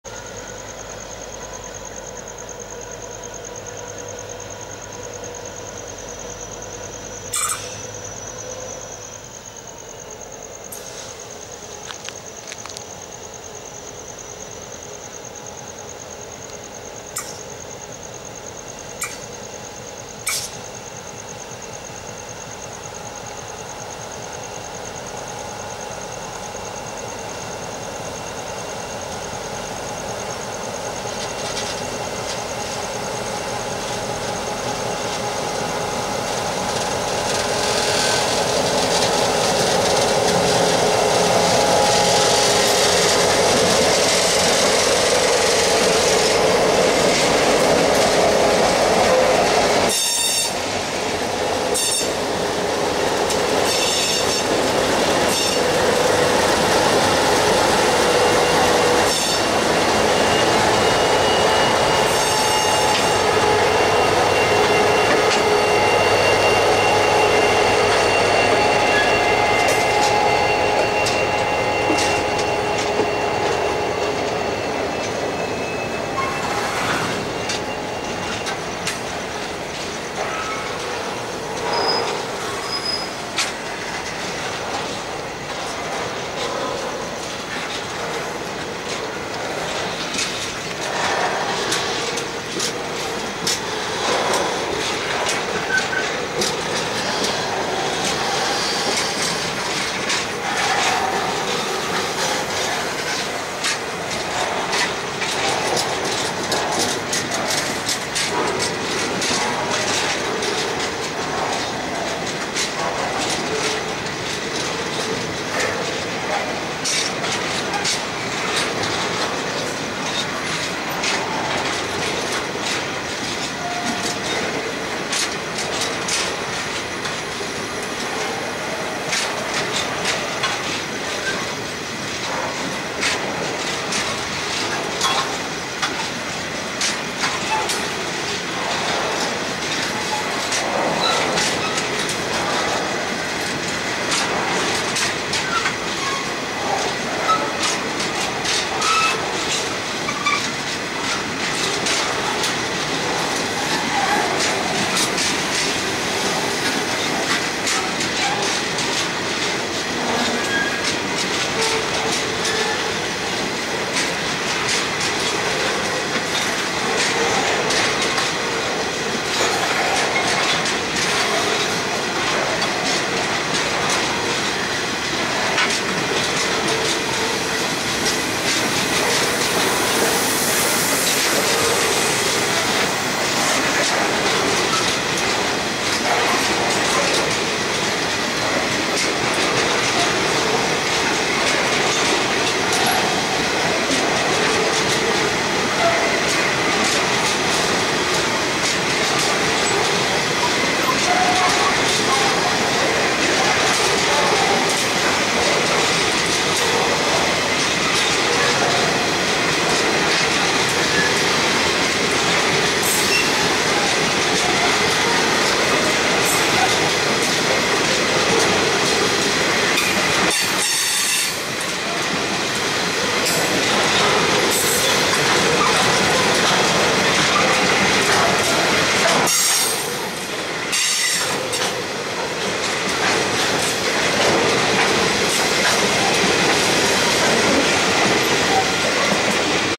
BRM001+602+48s35+C506=Werris Creek
SSR empty grain departing Werris Creek to Narrabri .
Werris Creek.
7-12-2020